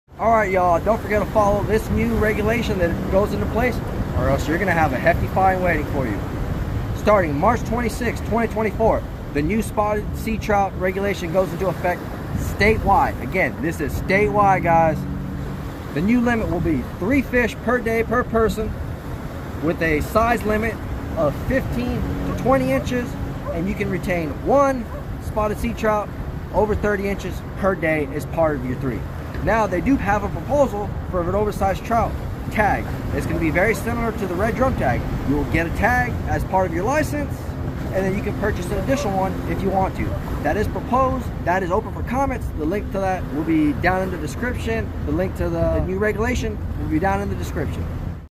NEW Trout Regulations TEXAS 2024
New Spotted Seatrout aka Speckled Trout aka Trout regulations.